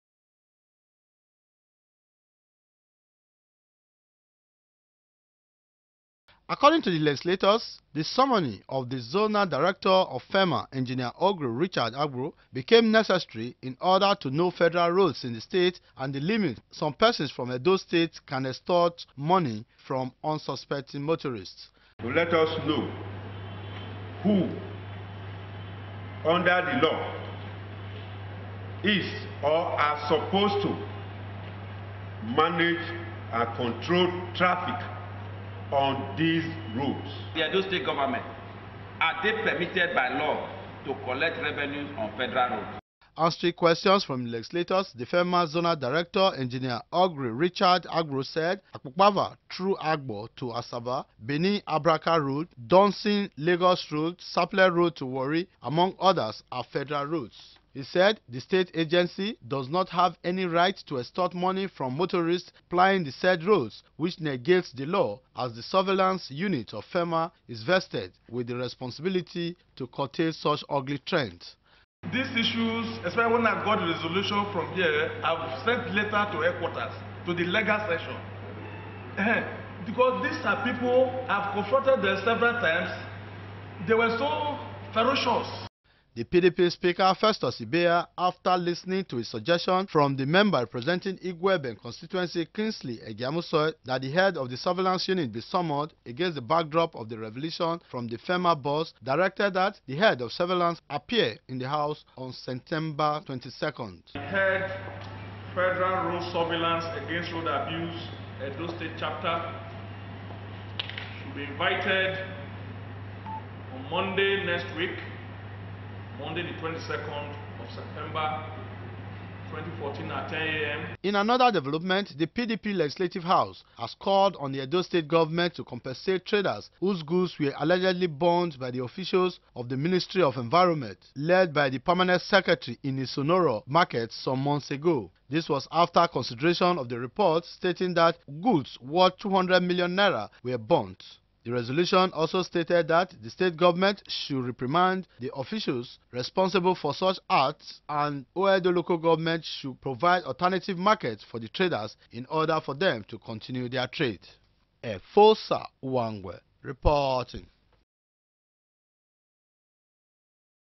PDP EDHA drills FERMA Zonal Director over Federal Roads
The Zonal Director of the Federal Road Maintenance Agency has appeared before the PDP faction of Edo State House of Assembly to explain why steps have not been taken to arrest some persons in the state who allegedly extort money from unsuspecting motorists on Federal Roads.